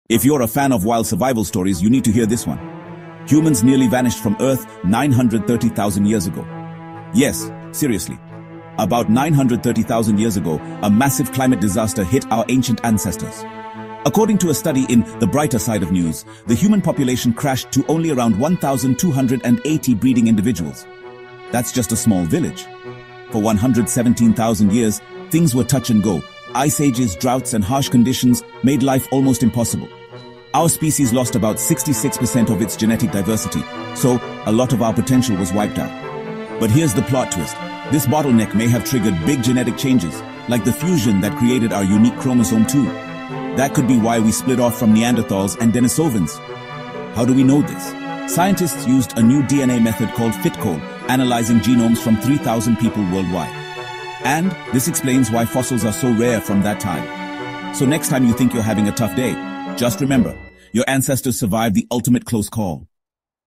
Humans Almost Went Extinct 930k Years Ago
Discover the astonishing revelation that nearly a million years ago, humanity faced extinction, with only about 1,280 individuals surviving a prolonged population bottleneck. This pivotal event, uncovered through advanced genetic analysis, offers profound insights into our evolutionary history and the resilience of our species